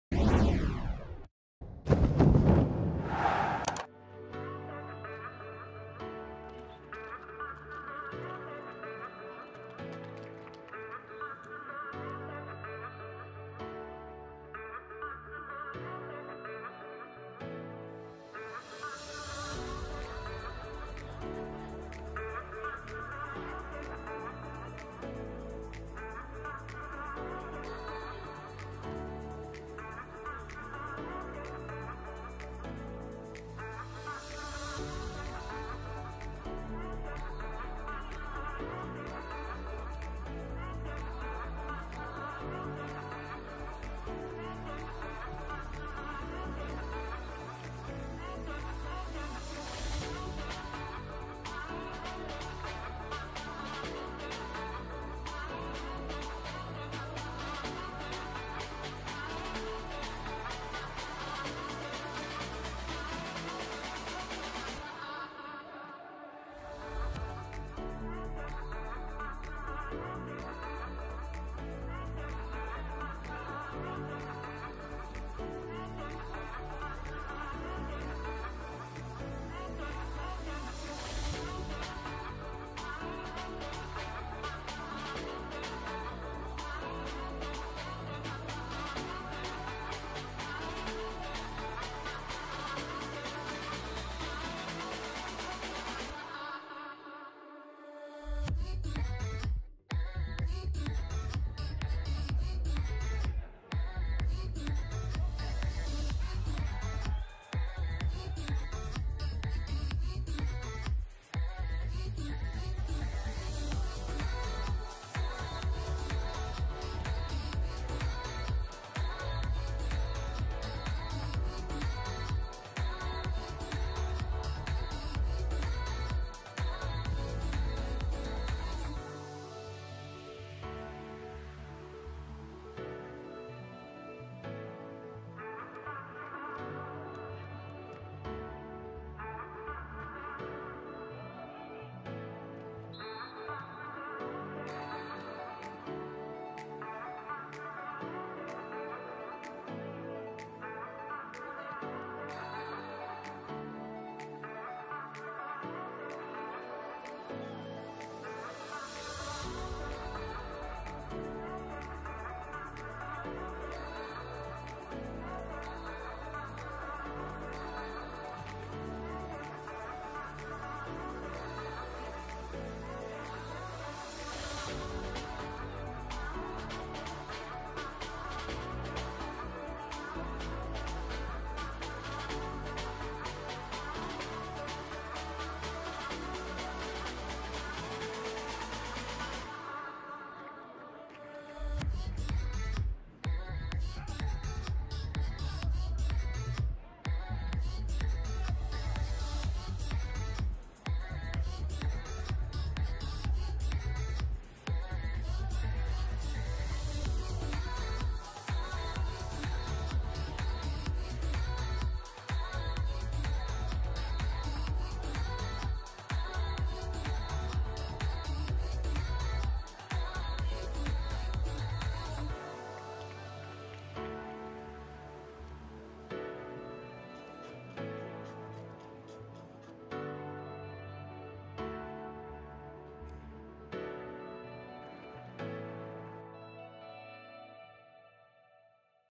Нёман (д) - Шахцёр С (д) / агляд / 06.03.2020
Нёман Гродна (дублёравы склад) - Шахцёр Салігорск (дублёравы склад) / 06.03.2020
Neman Cup 2020 сярод дублёравых складаў
футбольны манеж г.Гродна